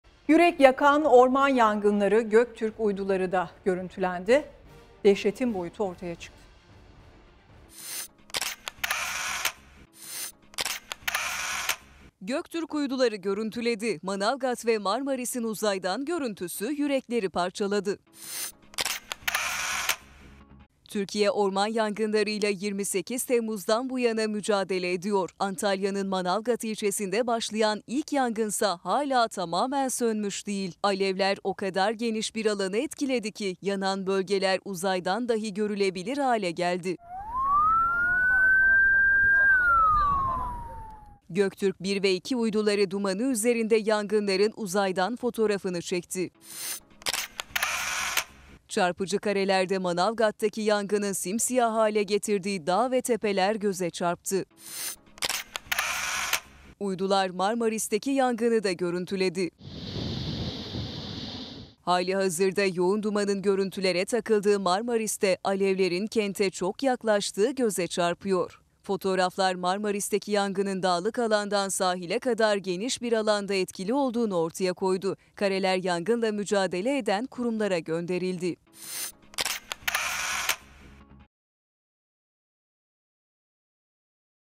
Göktürk Uyduları Acının Manzarasını Gözler Önüne Serdi: Ormanlar Simsiyah!
Türkiye'nin yüksek çözünürlüklü yerli keşif uydusu Göktürk-1 ve Göktürk-2, Manavgat ve Marmaris'teki orman yangınlarını görüntüledi.

Milli Savunma Bakanlığı'nın paylaştığı uydu fotoğraflarında yangının boyutu da gözler önüne serildi.

Yeşil ormanların yerini alan simsiyah bulutlar, acı tabloyu gözler önüne bir kez daha serdi.